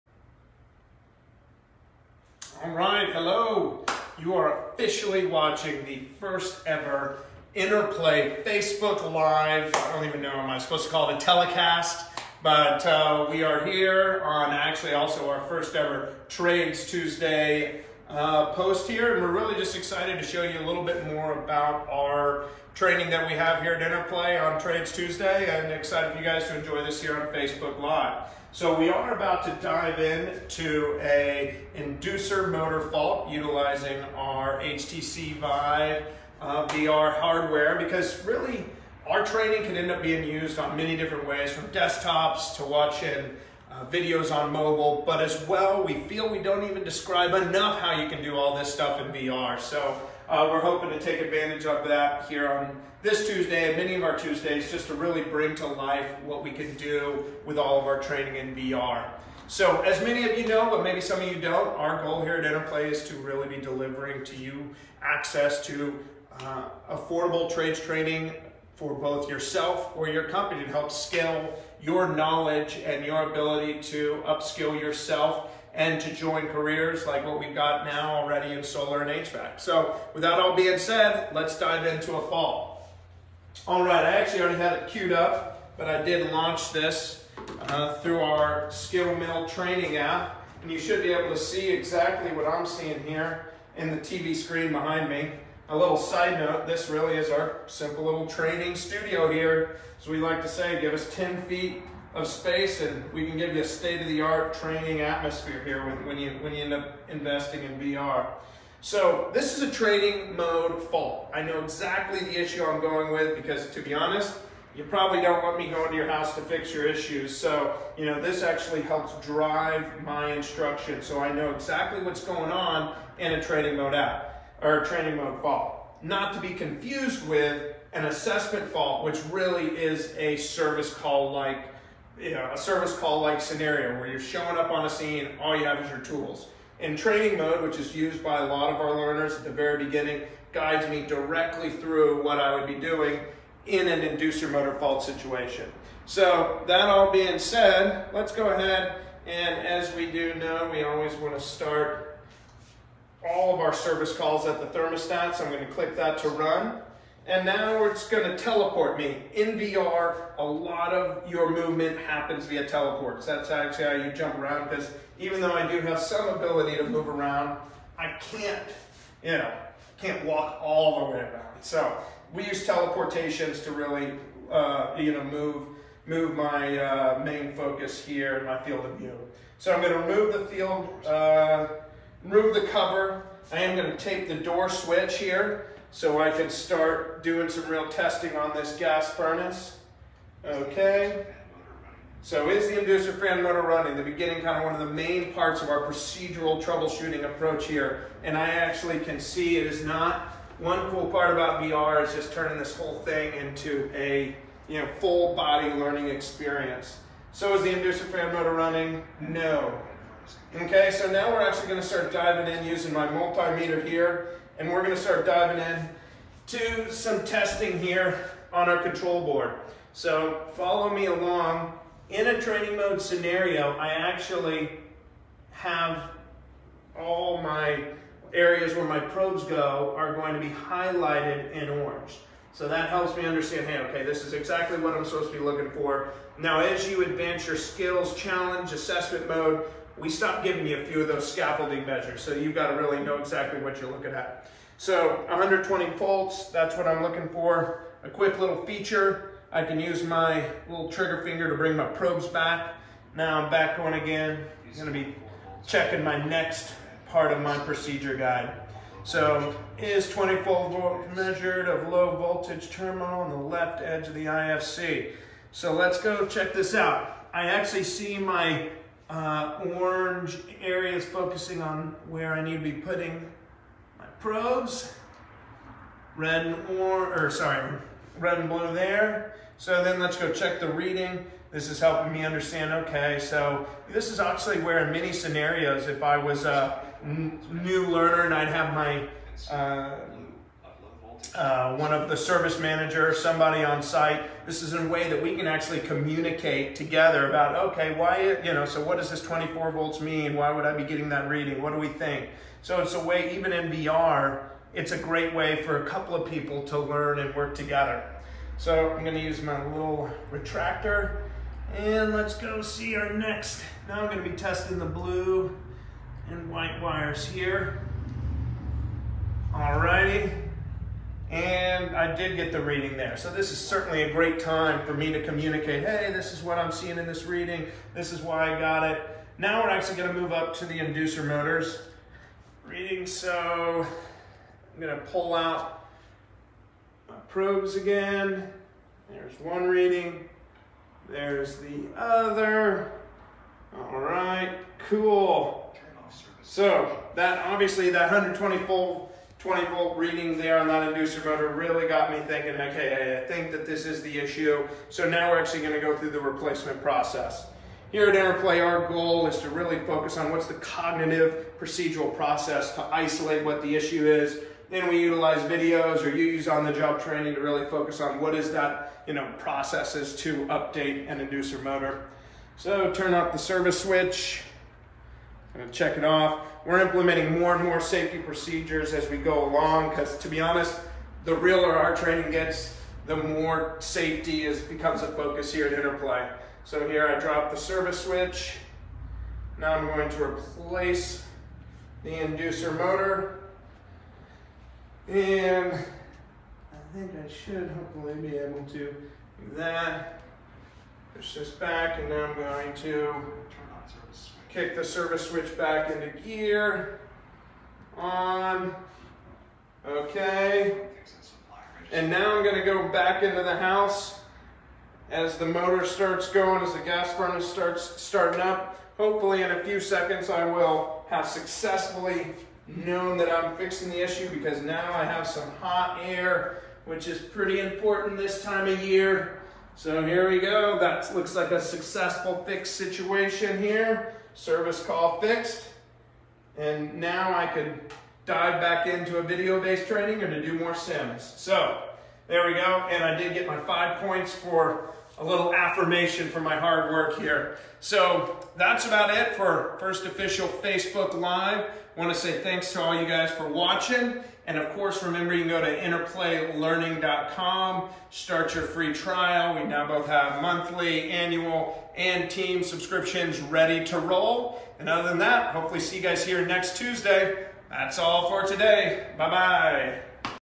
TradesTuesday: HVAC Training VR Walk-Through
Join Craig Carter, Interplay Learning's Sales Guru as he puts the "fun" in Fundamentals of VR training for HVAC.

For this tradestuesday, Craig walks you through you how to troubleshoot a Faulty Inducer Motor using Interplay Learning's 3D Simulation Training with VR enabled technology.